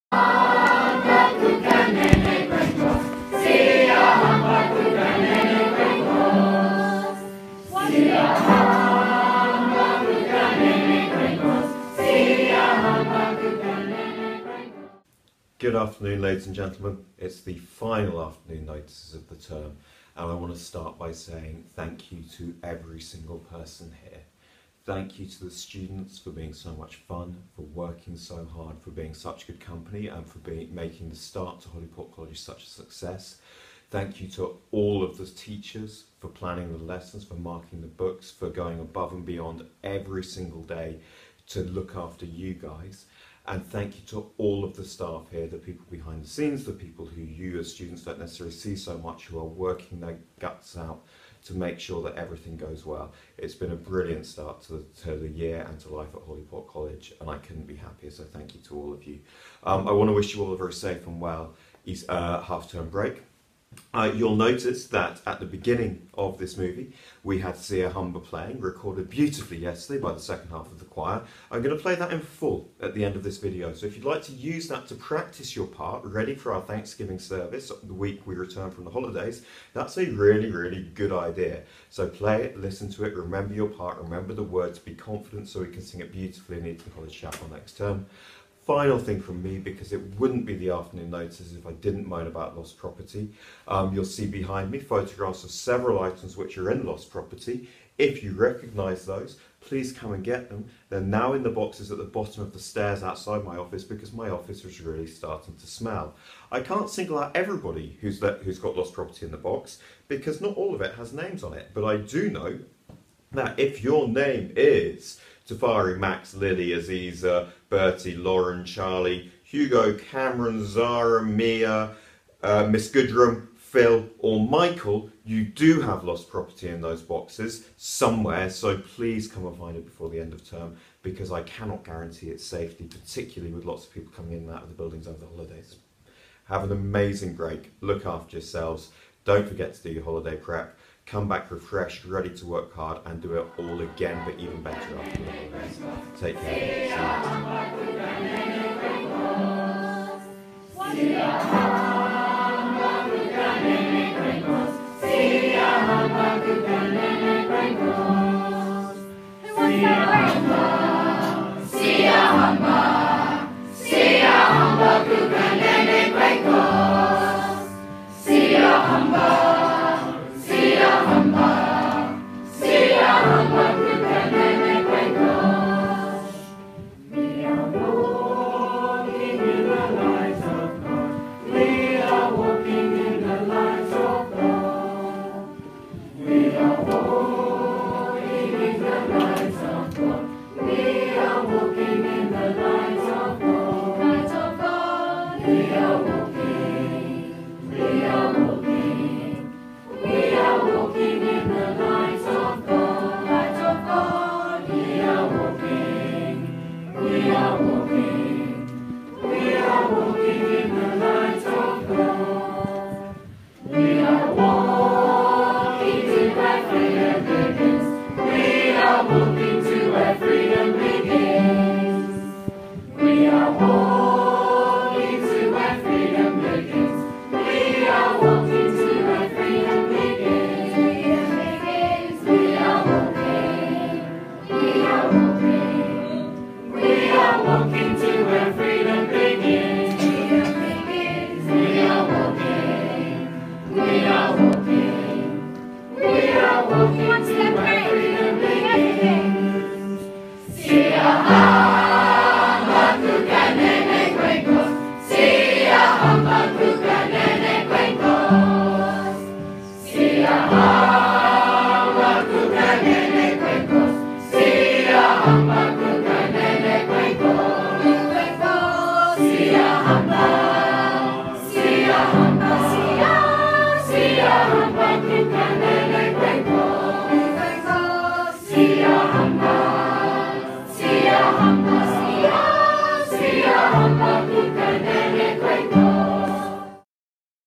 Notices 17th October
Notices 17th October- have a great half term and don't forget to practice for the thanksgiving service